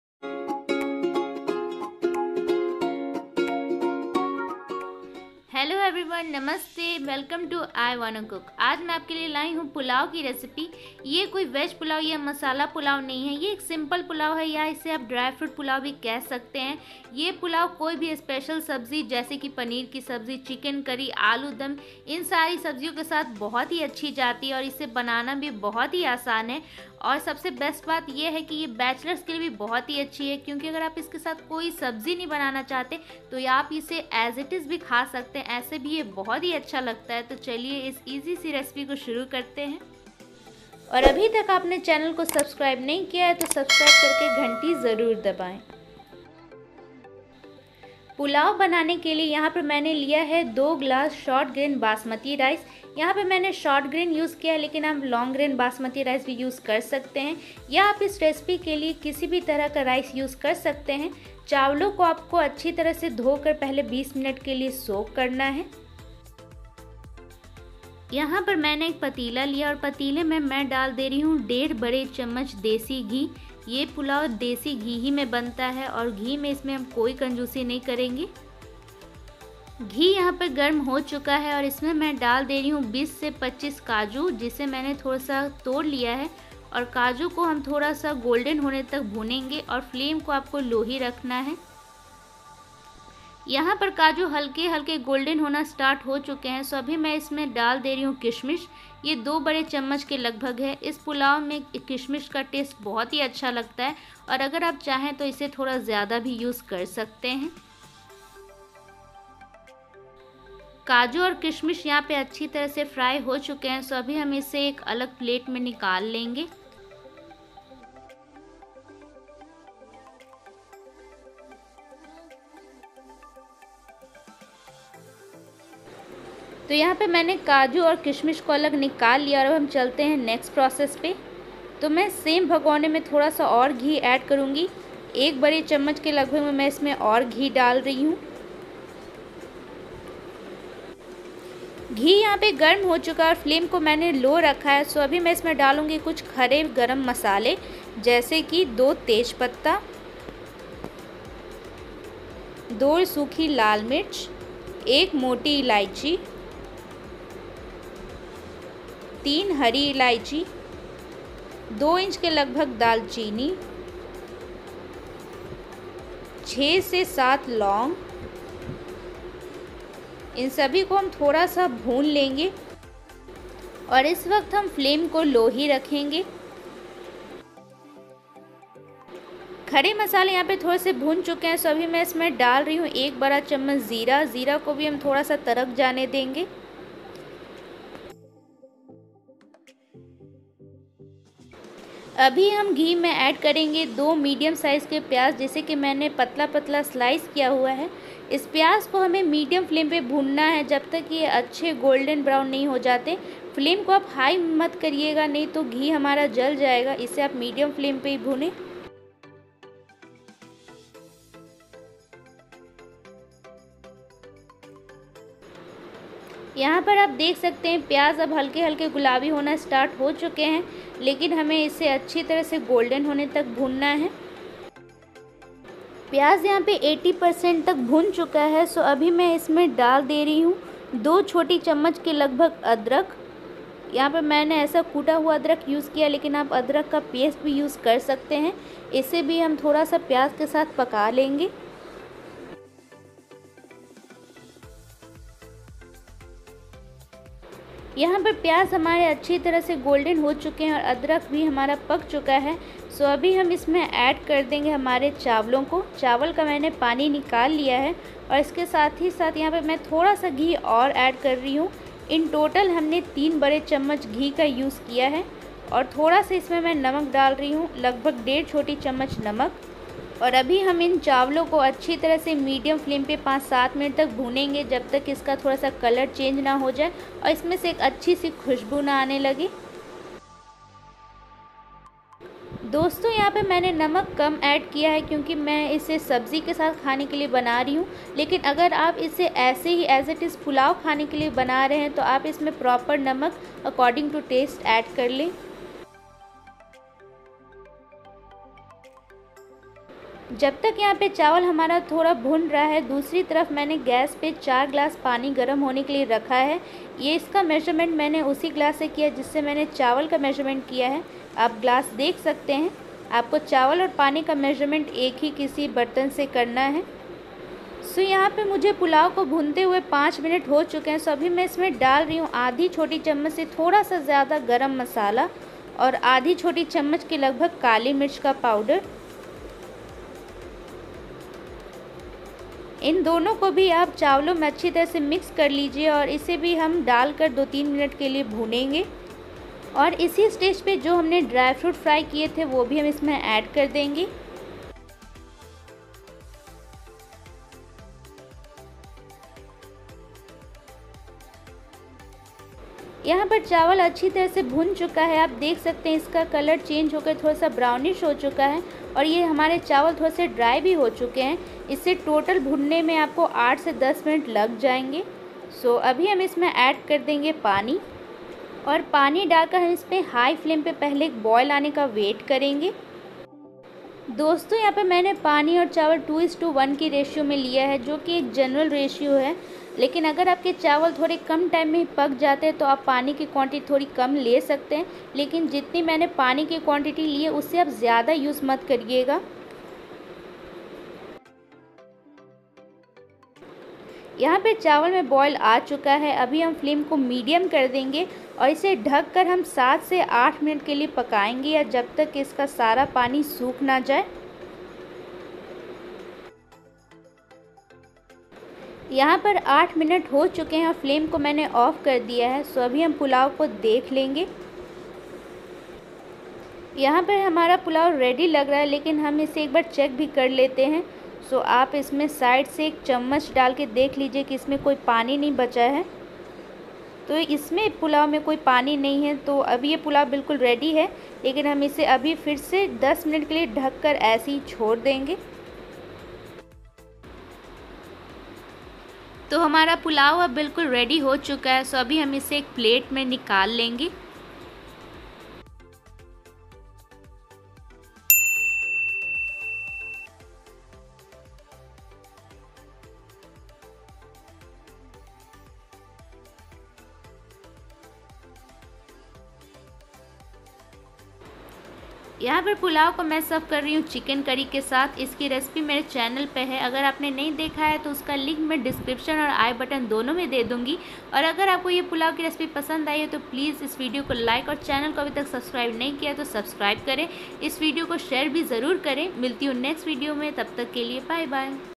Pulao recipe| Dry Fruit Pulao recipe| Easy Pulao recipe for bachleros| Indian Pulao | I Wanna Cook
Pulao /pulav is a integral part of Indian food. Any celebration or special day is incomplete without this. Isn't it? So here I am sharing with you all very easy and tasty recipe of pulao which you can enjoy with your special subzis like paneer, aloo dum, chicken, kathal, daal tadka etc. This pulao recipe is also great a great option for bachelors as they can enjoy this pulao as it is even without any subzi. Hope you all like this recipe.